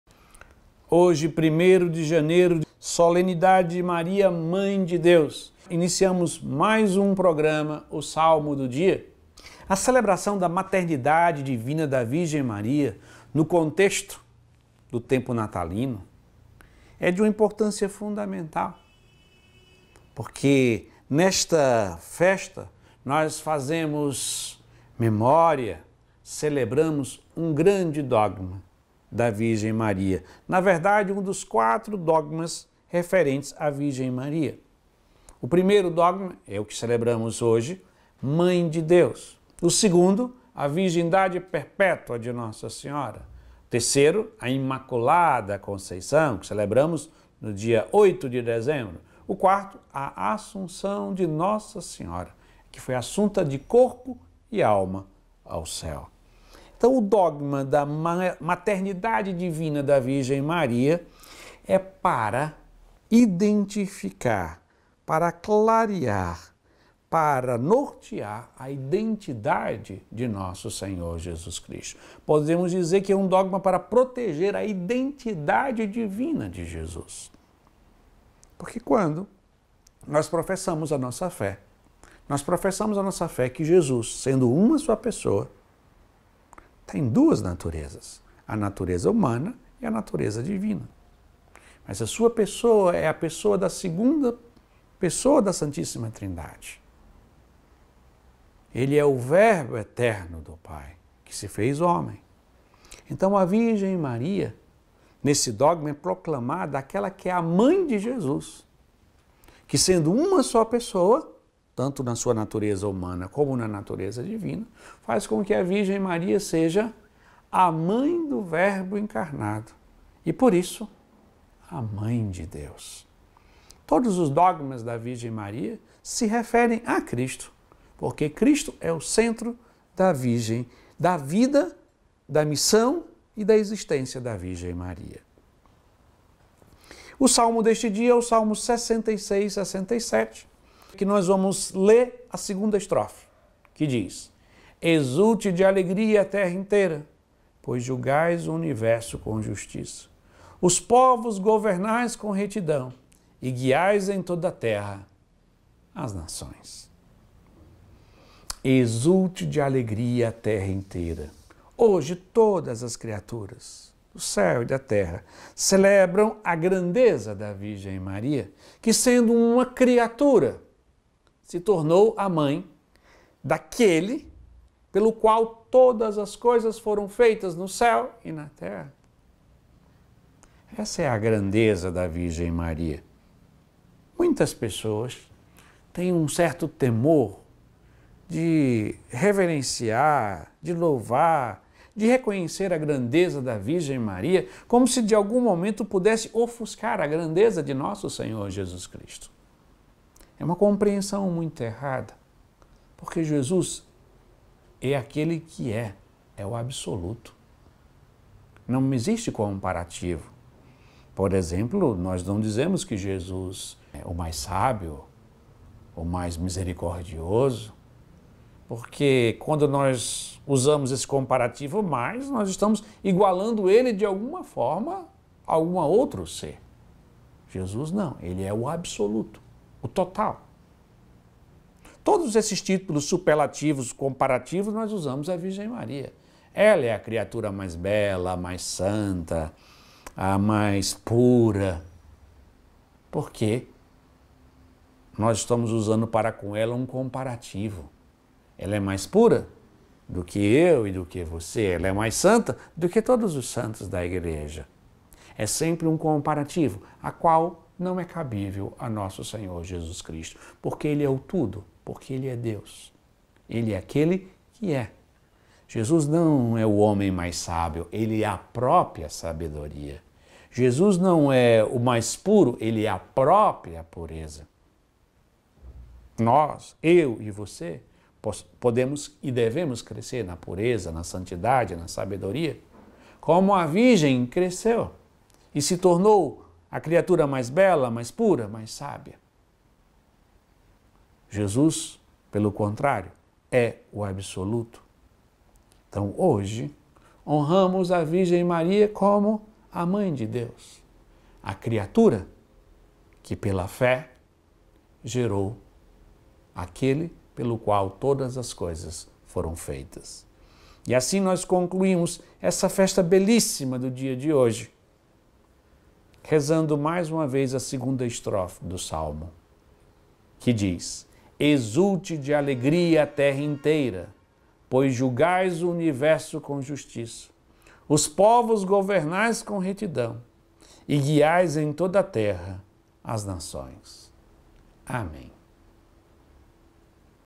Salmo do dia 01.01.2022 - Salmo 66 (67)
Exulte de alegria a terra inteira, pois julgais o universo com justiça; os povos governais com retidão, e guiais, em toda a terra, as nações.